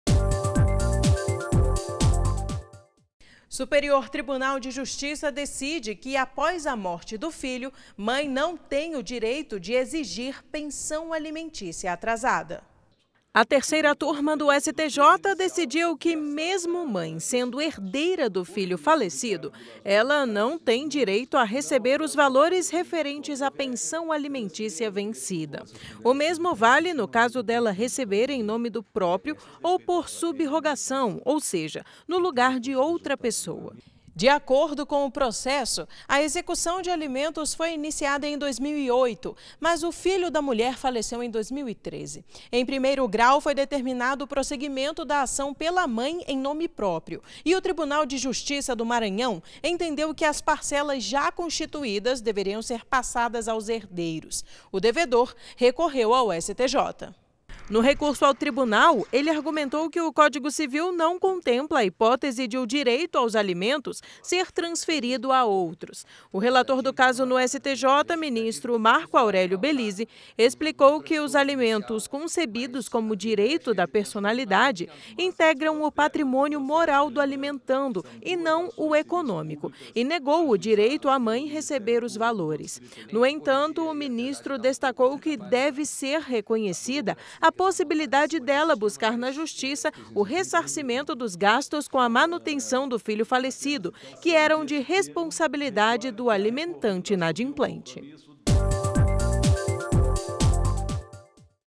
Clique STJ - Pensão Pós Morte (02/04/2019)
Superior Tribunal de Justiça decide que após a morte do filho, mãe não tem o direito de exigir pensão alimentícia atrasada.